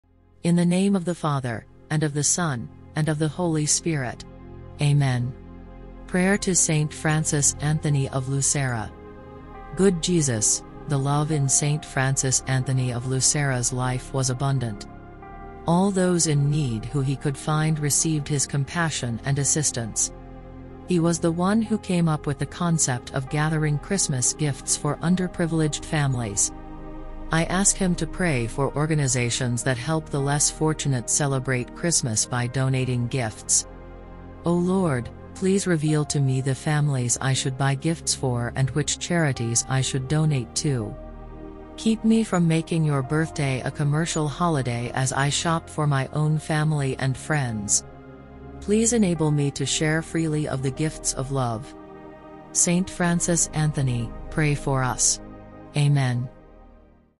Prayer to Saint Francis Anthony of Lucera
Born in 1681 at Lucera, southeastern Italy, Saint Francis Anthony Fasani was the son of very poor peasants; but he was a bright lad, and received a good education from the Conventual Franciscans in his native town.

When Saint Francis Anthony Fasani was 14 he received the habit of St Francis among the Conventuals, and in 1705 he was ordained to the priesthood. He was then sent to the Sagro Convento, adjoining the basilica in Assisi where St Francis is buried, for the purpose of continuing his studies. Two years later he received the doctorate in theology, and he was then appointed lector of philosophy in the college conducted by the Conventuals in his home town.

Saint Francis Anthony Fasani was promoted successively to regent of studies, guardian, and provincial, which latter office he held from 1721 to 1723. After that he served as master of novices, and then as pastor of the church of St Francis in Lucera. A bishopric was offered to him, but he declined it.

From the process of beatification we learn that Francis Anthony was diligent in study, fervent in piety, prompt in his obedience to his superiors, devout in meditation, and most exact in the fulfilment of all his duties. He was also mortified and given to the exercises of penance even to bloodshed. From his youth he was an "angel in the flesh, more an angel than a man."